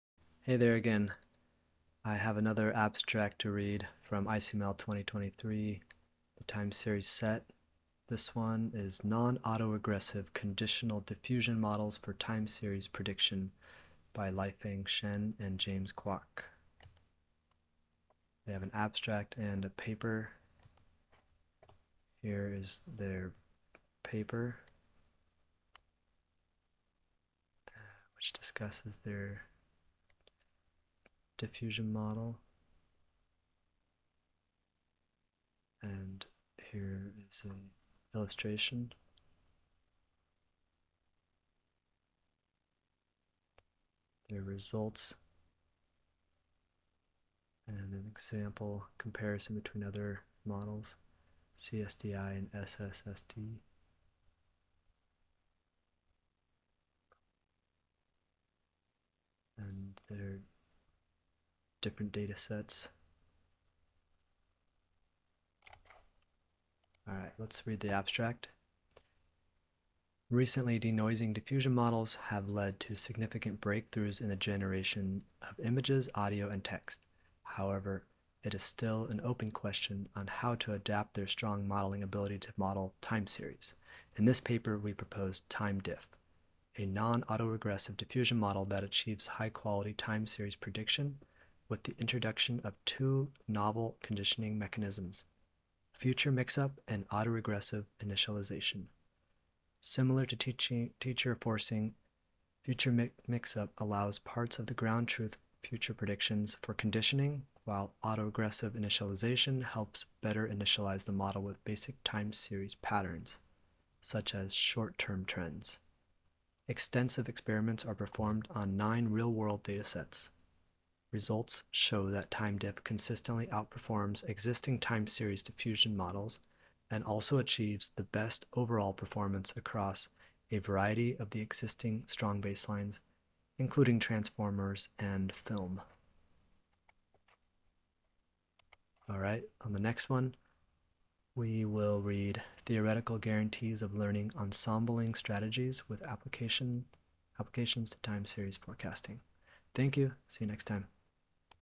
ICML AI - Non-autoregressive Diffusion Models for Time Series (5/15)
Listen to ICML 2023 AI/ML abstract "Non-autoregressive Conditional Diffusion Models for Time Series Prediction" by Lifeng Shen and James Kwok.

Supporting your education and research: